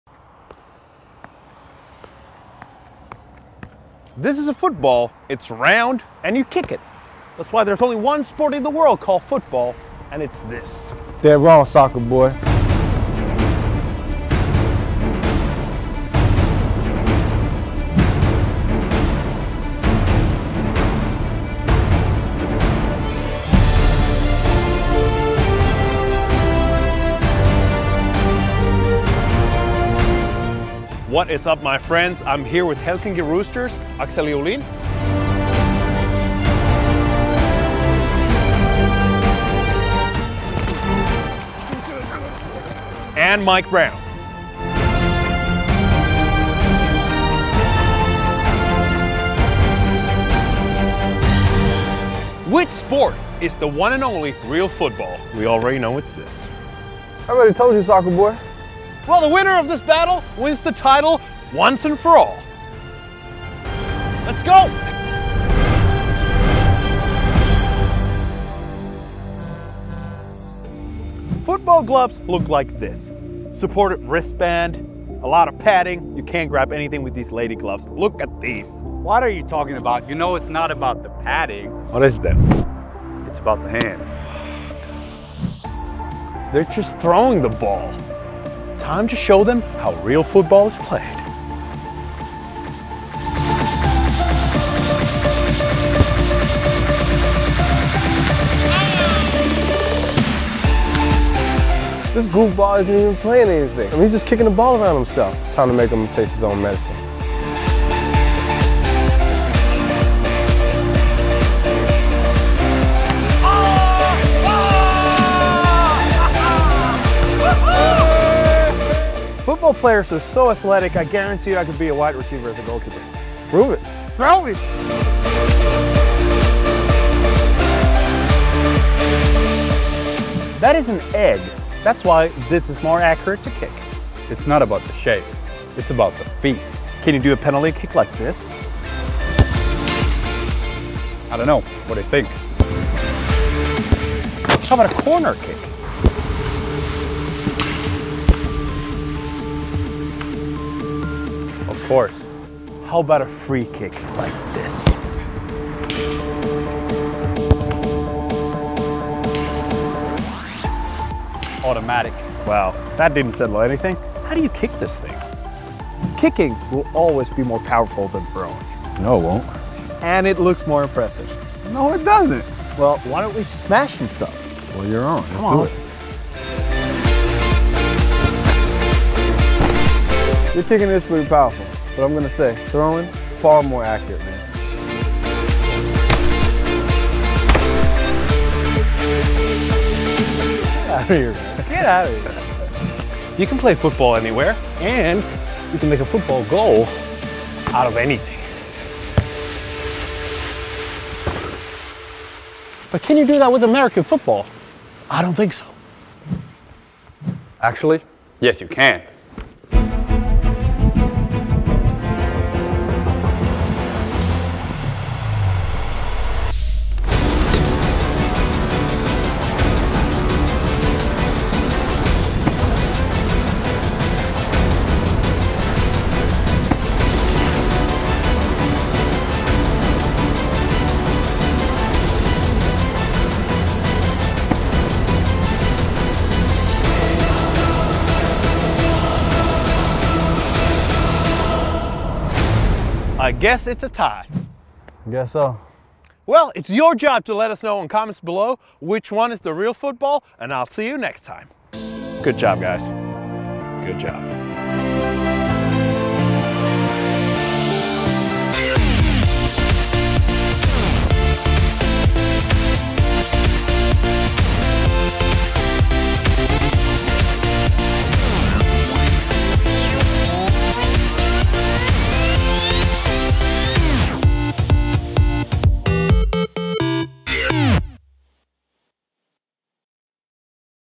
Football vs. Football | Which one is the REAL FOOTBALL? | Lassi Hurskainen vs. Helsinki Roosters
In this episode Lassi goes head-to-head with Helsinki Roosters' players Akseli and Mic. They are attempting to solve the biggest mystery of sports history: Which one is the REAL FOOTBALL - American or European? We would love to hear what you think.

Huge thanks goes to Helsinki Roosters for making this possible.